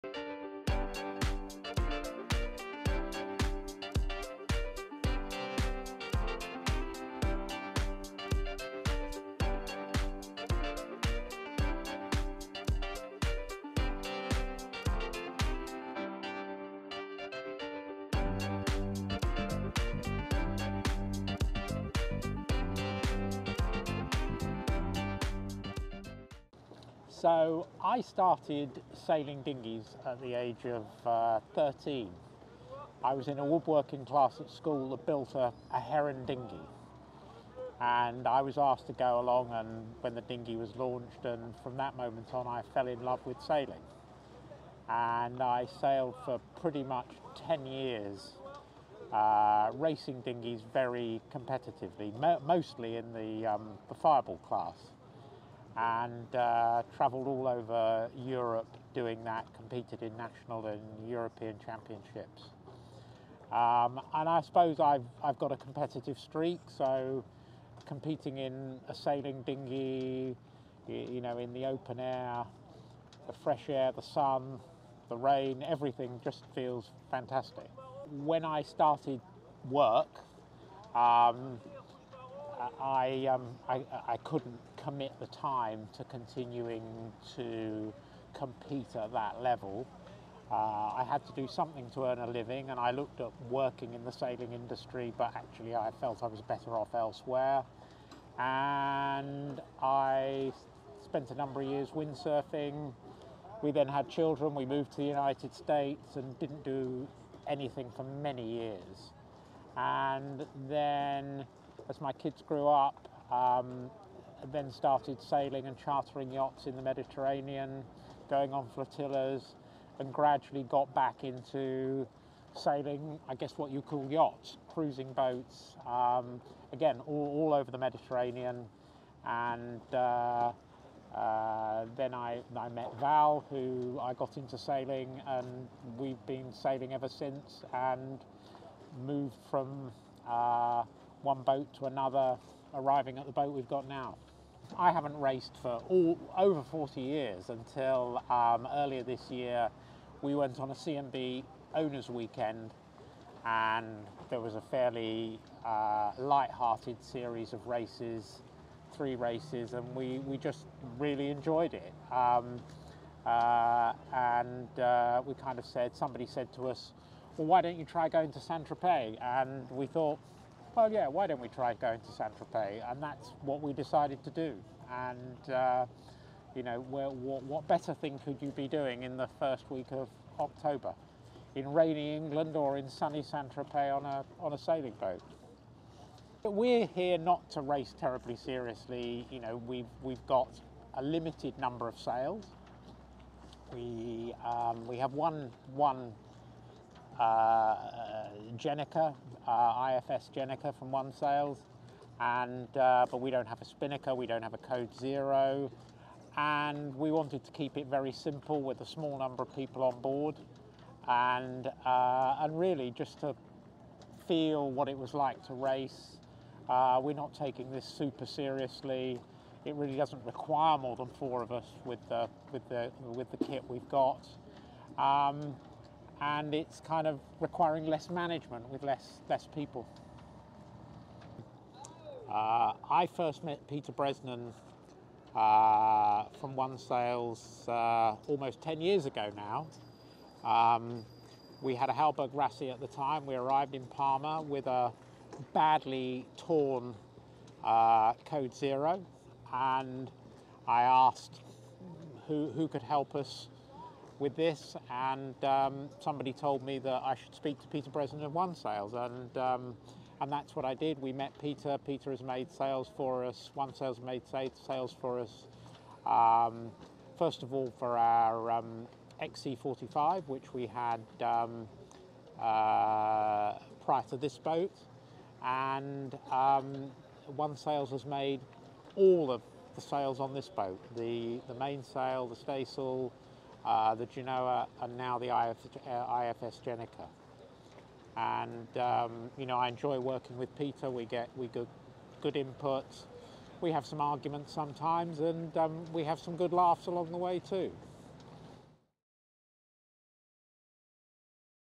Peter's Return to Racing at Les Voiles de Saint Tropez
Welcome back to RegattaAdventures! In this episode, we're again off to Les Voiles de Saint Tropez, where four amazing yachts were sailing fully equipped with OneSails sails. We sailed and had a sincere and natural chat with boat owners. One remarkable person we met is Peter Bamford, the owner of the CNB 60 yacht 'Pat Bon.' Peter's love for sailing goes way back to his childhood when he competitively raced dinghies. Despite taking a break, his passion for sailing never waned. Today, he's back in the racing scene, competing in the Maxi class.

What makes Peter's story even more fascinating is that in Les Voiles de Saint Tropez, he sailed in the Maxii class with a crew of only four people, which is quite unusual. And we asked him why. Join us as we explore Peter's remarkable journey and his return to the racing world in this episode.